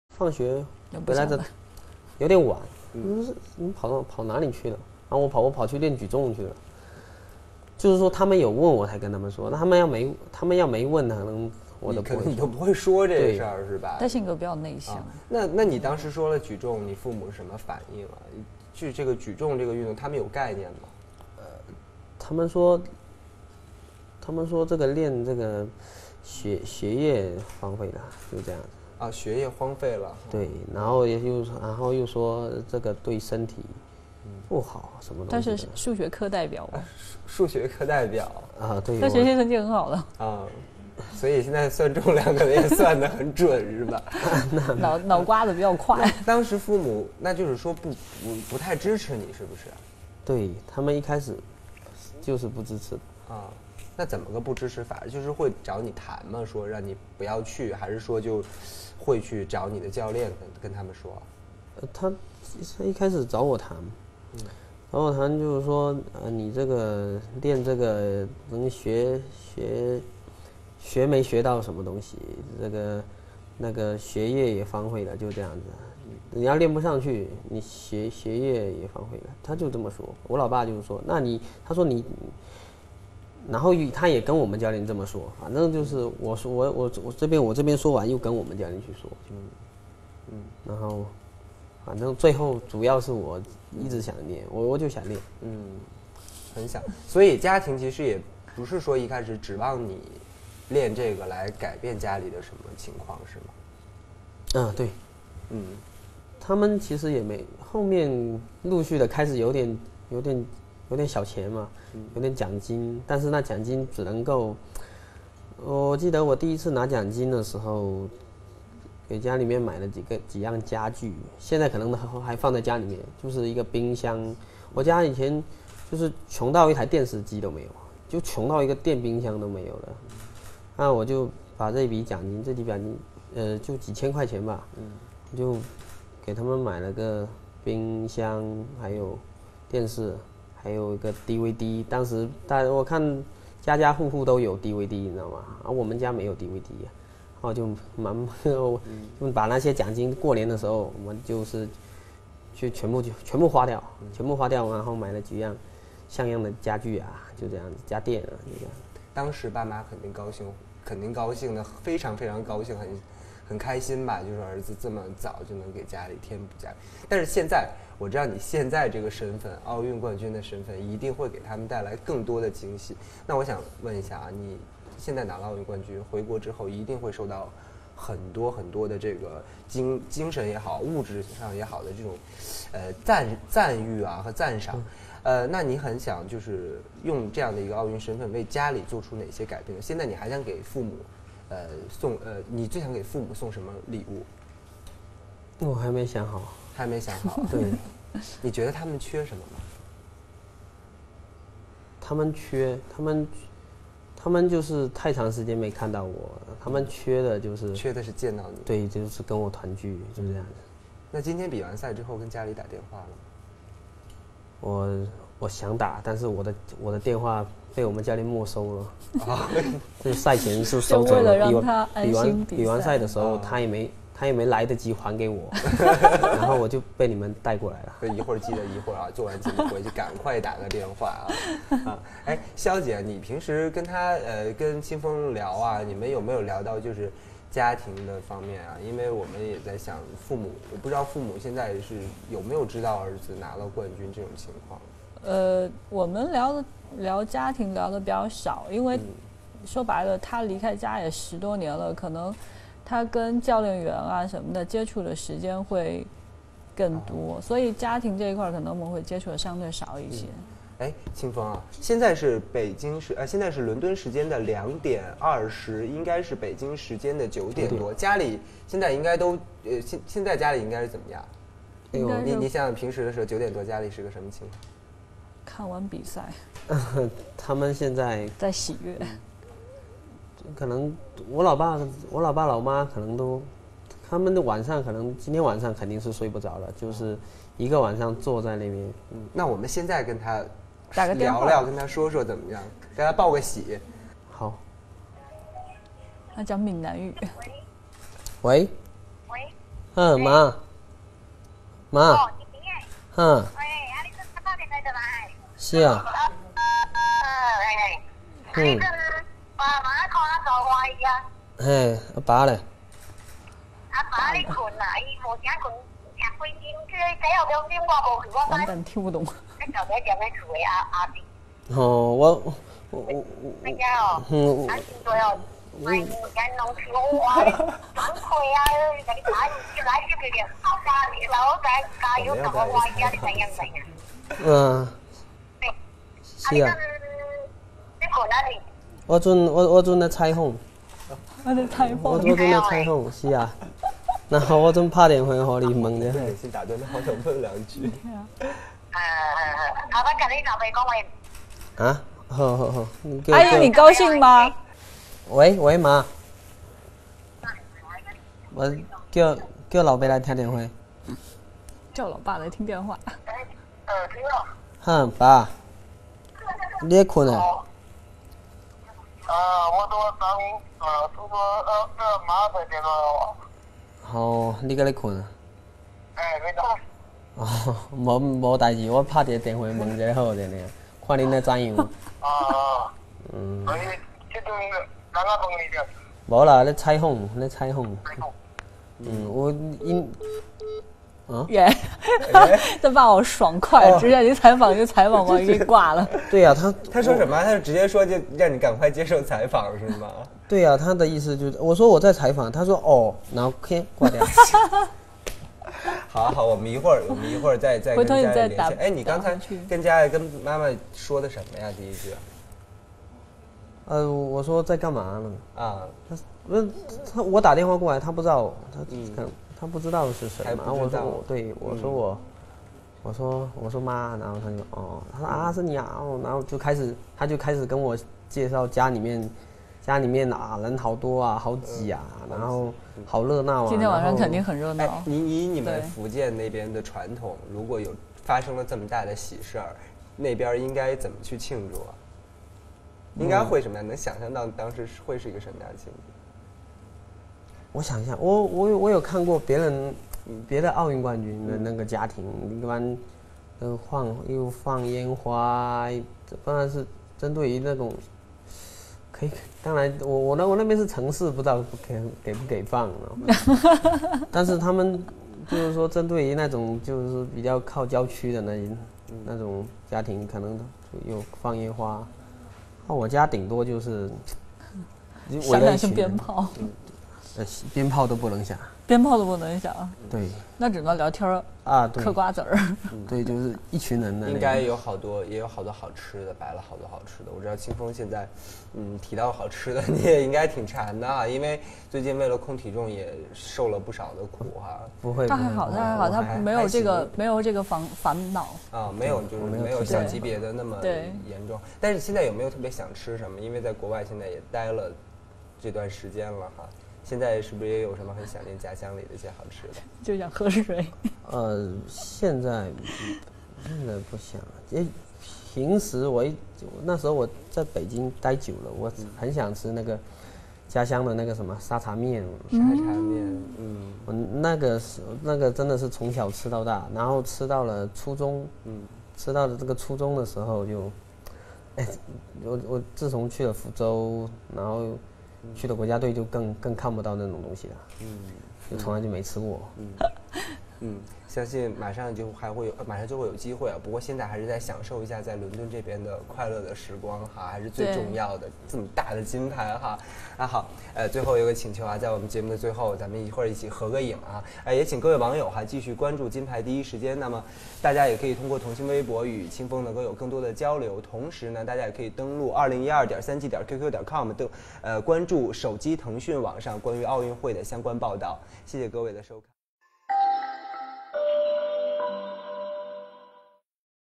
London 69kg champion Lin Qingfeng Interview Part 5
Lin: I got home really late and they asked me where I had been. I told them I had been weightlifting. If they hadn’t asked him, I would never have told them.
Q: What was their response? Did they have any idea what weightlifting was?
Lin: They told me that it did nothing for my education and that it was bad for me physically.
Other Reporter: Lin was the math representative of his class. He had really good grades.

Q: So they didn’t support you?
Lin: They were not supportive. They would sit me down to talk with me. They asked me if I learned anything from this? My dad said that if I didn’t make it to the top, my education would be wasted. He told this to me and my coach. But I liked it so I stuck with it.

Q: So they never expected you to make anything of it?
Lin: Correct. But later on I started to win some money from my competitions. I remember the first time I won money, I bought some new furniture for our home. We were so poor we didn’t even have a TV or a fridge. So with the few thousand yuan, I bought a TV, a fridge, and a DVD player. I noticed that every home had a DVD player and our’s didn’t. During the new year, I made it a goal to spend all of the prize money.

Q: Now that your olympic champion, what kind of gift would you like to give your parents?
Lin: I’m not sure.
Q: What do you think they are missing?
Lin: I guess its been a long time since they saw me. A reunion would be nice.
Q: Have you called your parents after your victory?
Lin: No. My phone was confiscated by my coach before the competition. He didn’t get the chance to give it back to me yet. And you guys dragged me here immediately afterwards.

Q: Its currently 2:20 am London time, what do you think your folks are doing back at home?
Lin: I don’t think they would be able to sleep tonight?
Q: Lets call them right now then hows that?
Lin: Sure.

*Lin calls his family. Can’t translate because I do not understand his hometown dialect. Lin’s dad apparently hung up the phone after he said he was being interviewed, presumably to not distract from his son’s spotlight.

Q: What was your mother saying?
Lin: She was describing he scene back at home. Apparently our apartment was packed. A lot of people were over to watch the competition.

Q: Do you miss ay particular food from home?
Lin: When I was training in Beijing, I would crave Sha Cha noodles from back home. I was raised on it.